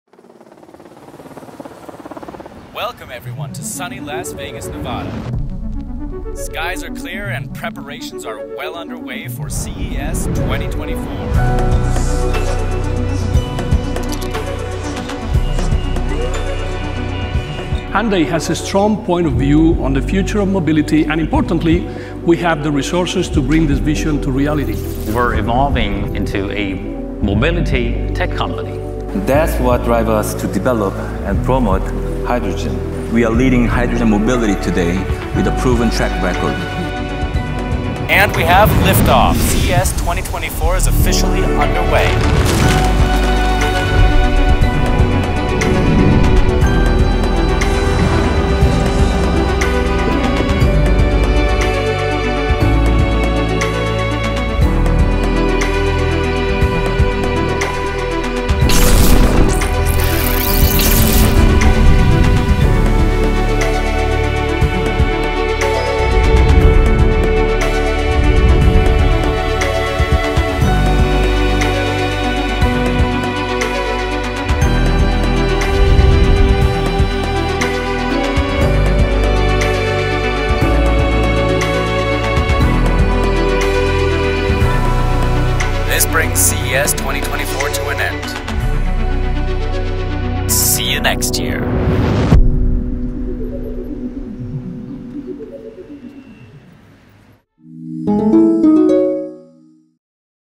새로운 미래를 그린 현대자동차의 CES 2024 현장 스케치 | 현대자동차
현대자동차는 복잡하고 오염된 도시에서 해방되어
깨끗하고 근심 없는, 편안함으로 가득한 기분 좋은 일상을 그려봅니다.

CES 2024에서 현대자동차는 수소 사회와
끊임없이 발전하는 소프트웨어가 바꿔 놓은 인류의 삶에 대한
새로운 청사진을 소개했습니다.

현대자동차가 그리는 미래를 CES 2024 부스 현장 스케치 영상을 통해 만나보세요.